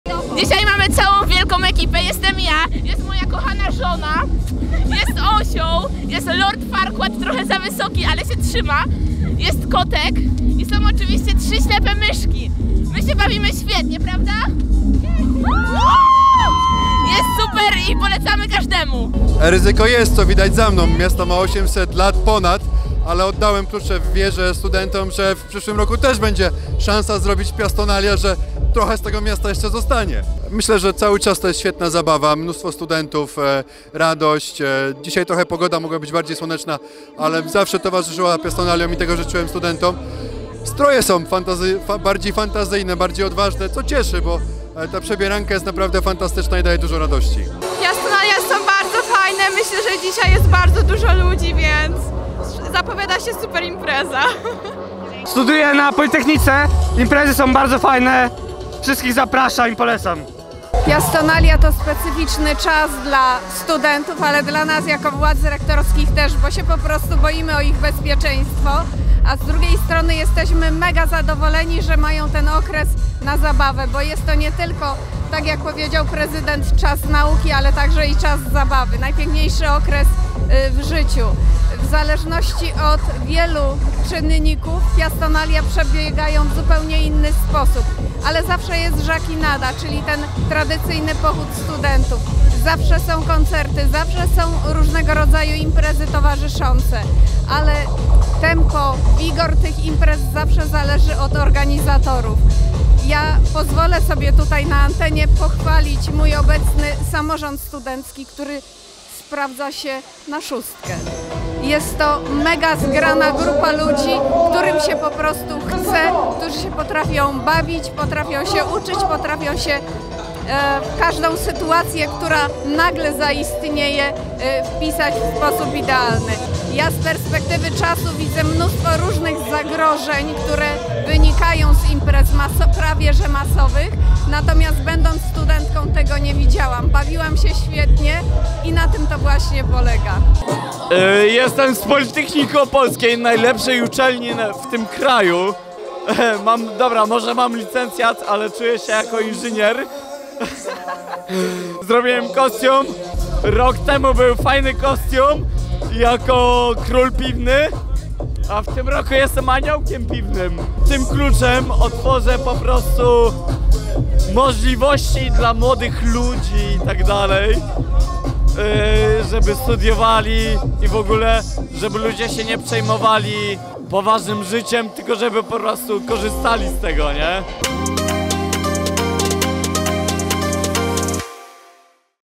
Piastonalia 2023 rozpoczęte!
Piastonalia START! Dzisiaj Prezydent przekazał studentom symboliczny klucz do bram miasta, rozpoczynając tydzień imprez studenckich. Już dzisiaj widać było, że żakowie opolskich uczelni bawią się świetnie, a to dopiero początek !